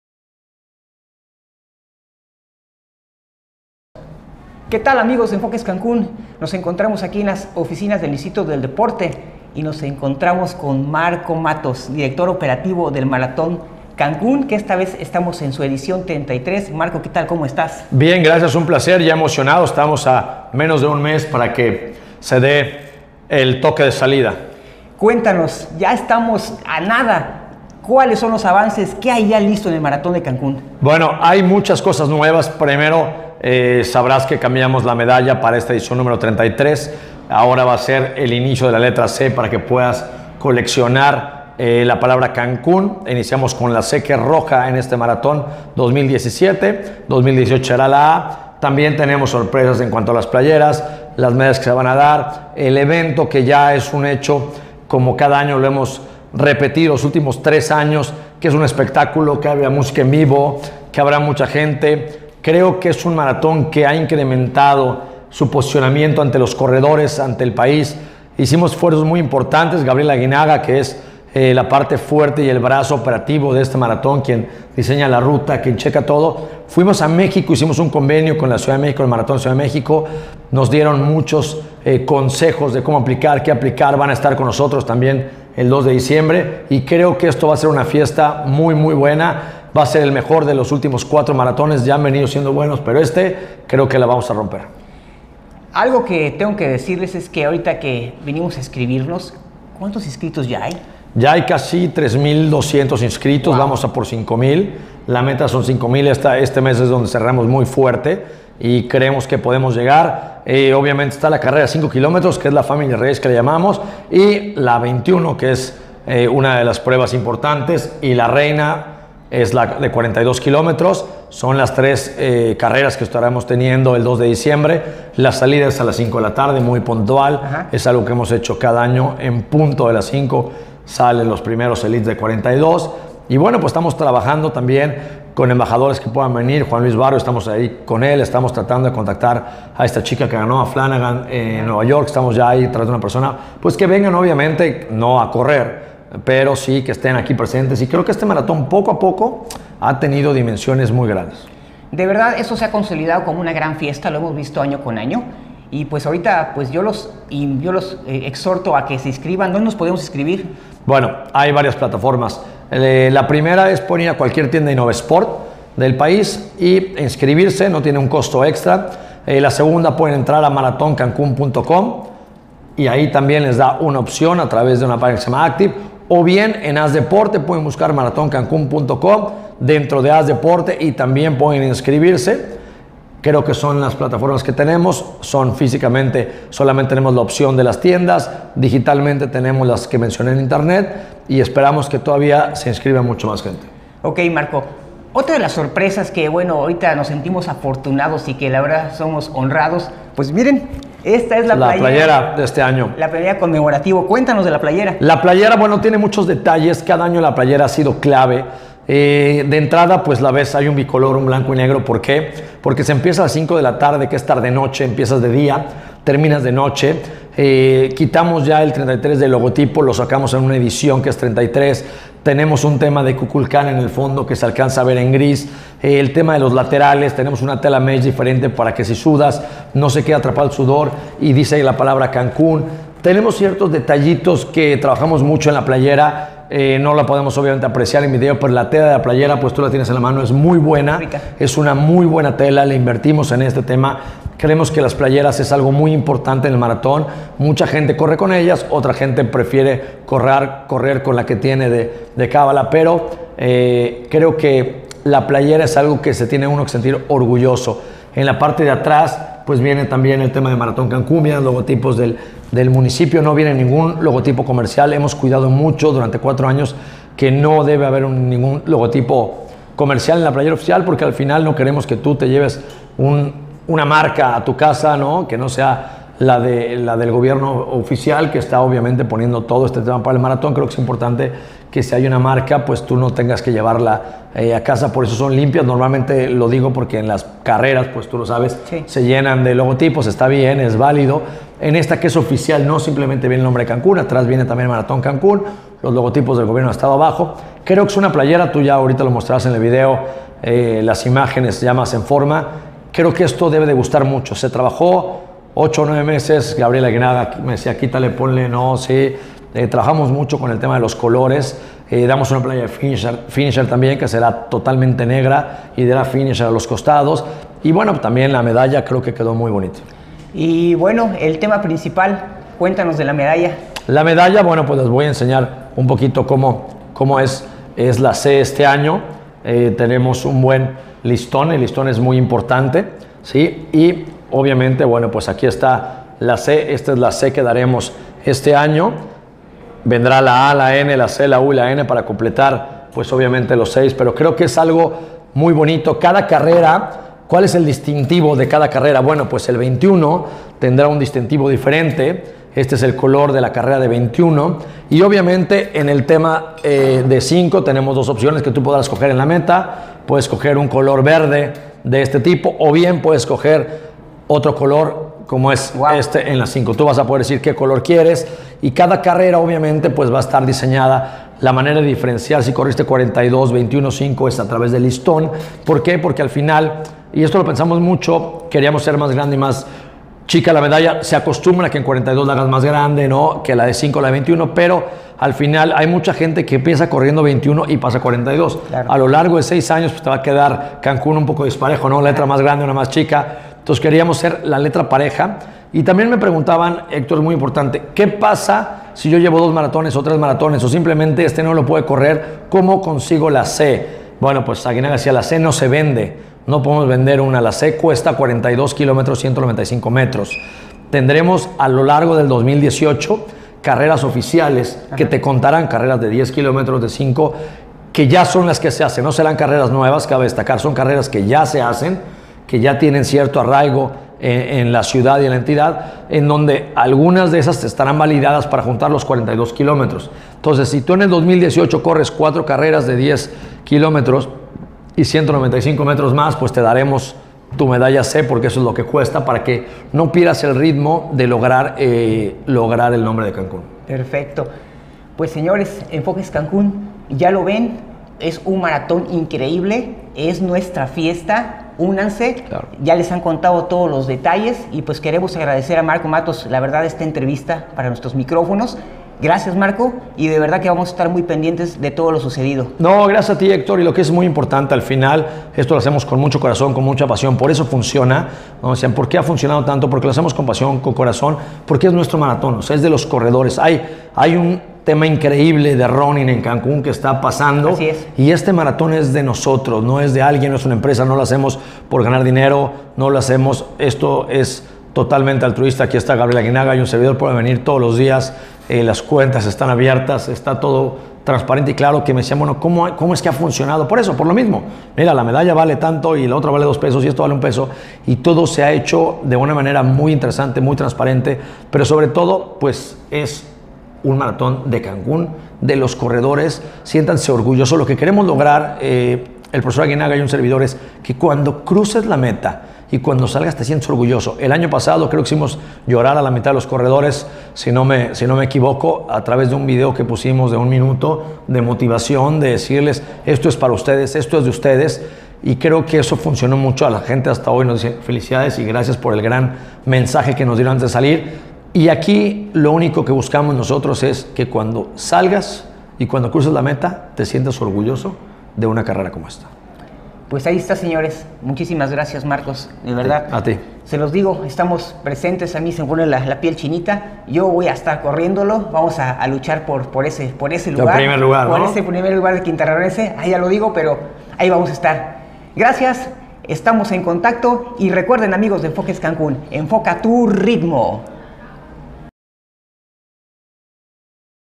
Marco Martos nos platica del 33 Maratón Cancún
Marco Martos nos platica del 33 Maratón Cancún entrevista por Héctor Beristain de